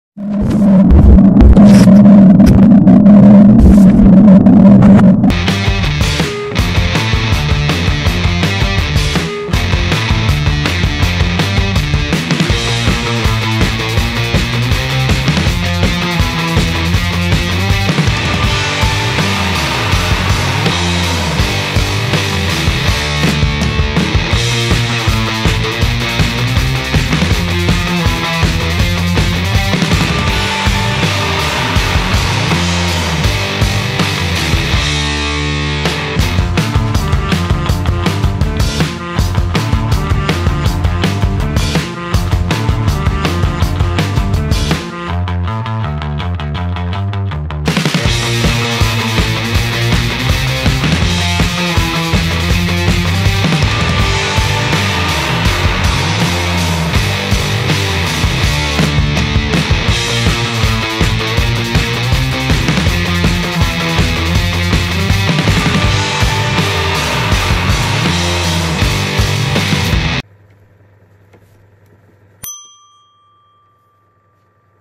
Geometric Tapir Drawing - Time Lapse
Quick video showing how I drew a Tapir.